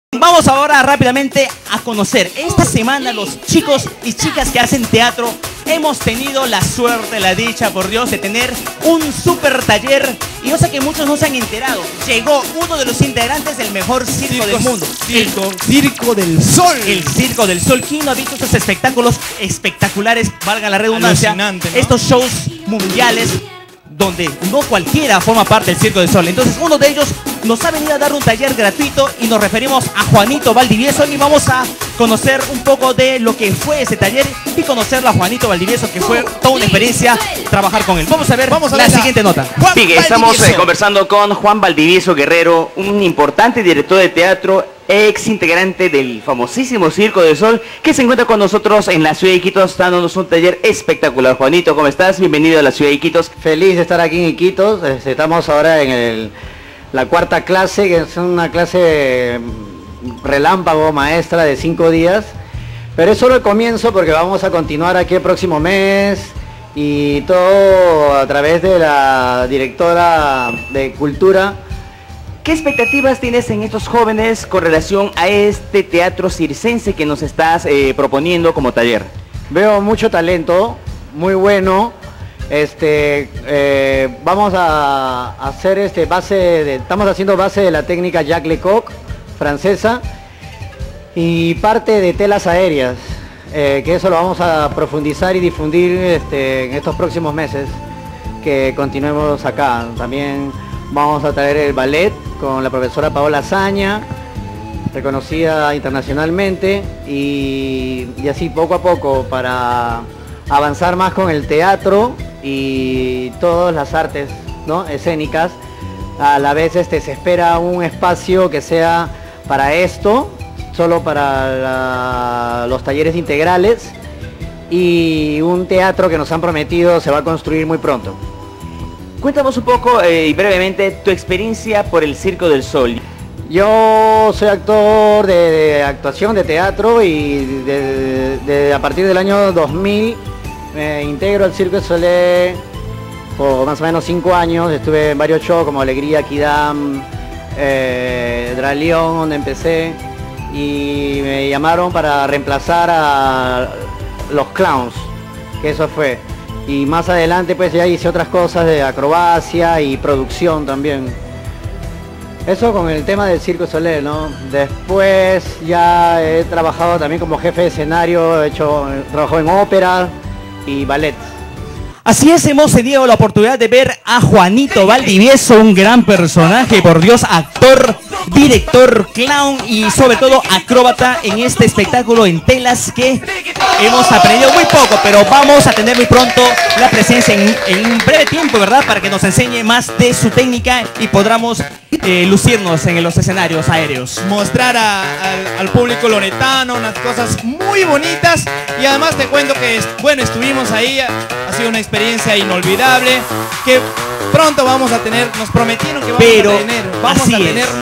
juan valdivieso
Ex integrante del conocido y afamado Circo del Sol, llega a Iquitos el actor Juan Valdivieso a darnos un taller relámpago a todos los actores de nuestra ciudad. En esta nota conoceremos un poco de su trayectoria y de las expectativas que tiene de los actores de nuestra ciudad.